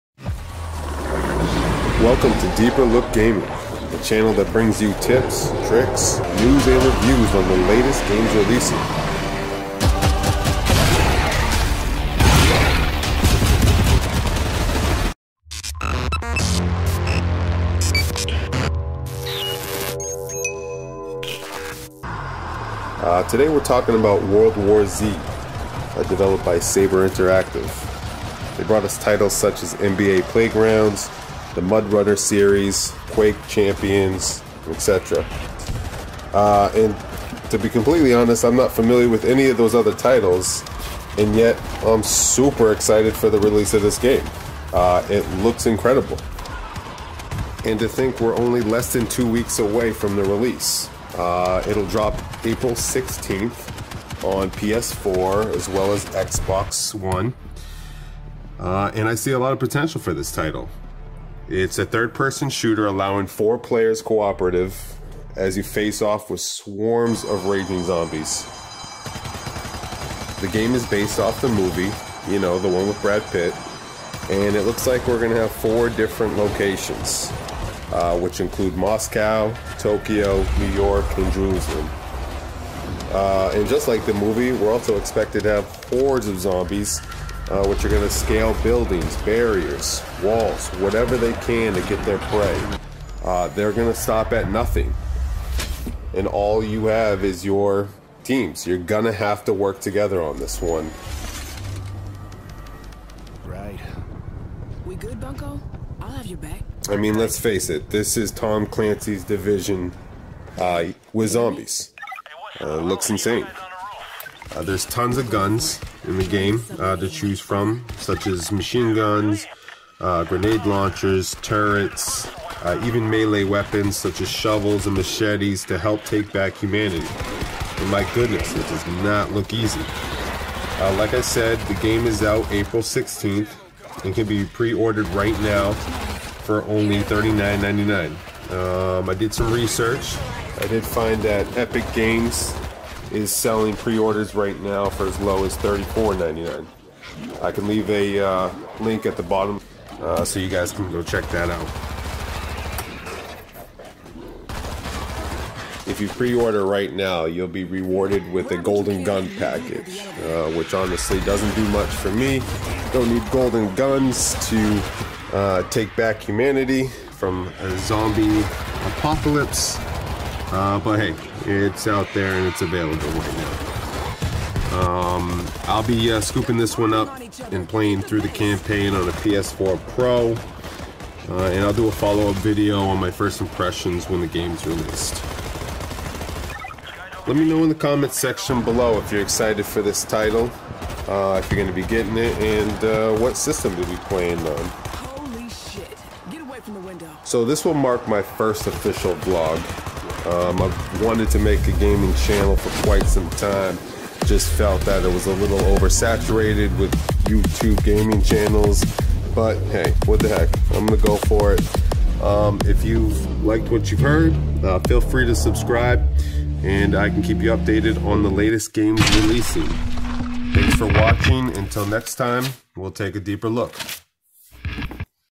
World War Z The Game_ Releasing April 16th 2019!
World War Z the Game will be real released on PS4 and Xbox One on April 16th by Developer Saber Interactive. A third Person, 4 player co_op that allows players to annihilate swarms of Zombies in order to take back the human race.
Based on the movie World War Z the game looks like a fun yet challenging fast pace shooter that I hope keeps us coming back for more. Look forward to hearing your thoughts on this game.